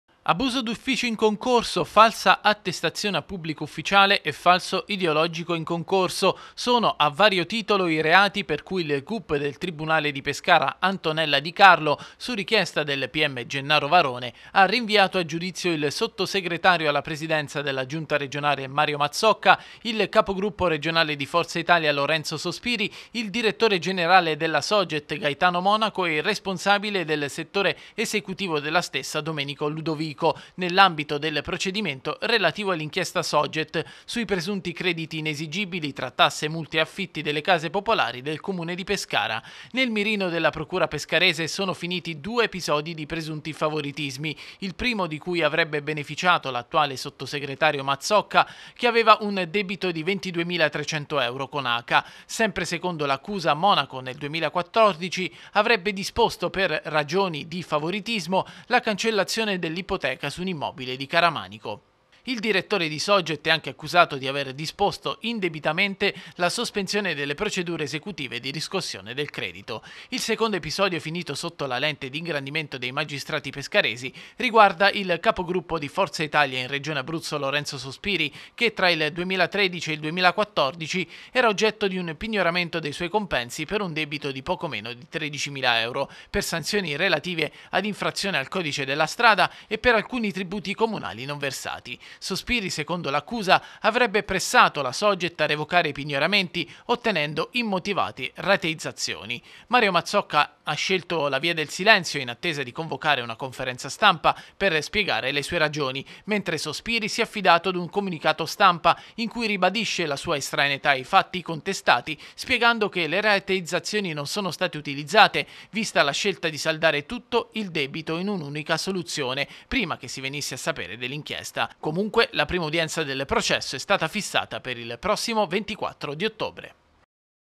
Inchiesta Soget Sospiri e Mazzocca a processo
Si andrà in aula il prossimo 24 ottobre per il dibattimento dell’inchiesta Soget, sui presunti crediti inesigibili del Comune di Pescara. Ieri il Gup del Tribunale di Pescara ha rinviato a giudizio 4 persone tra cui il sottosegretario alla giunta Regione Mario Mazzocca e il capogruppo di Forza Italia Lorenzo Sospiri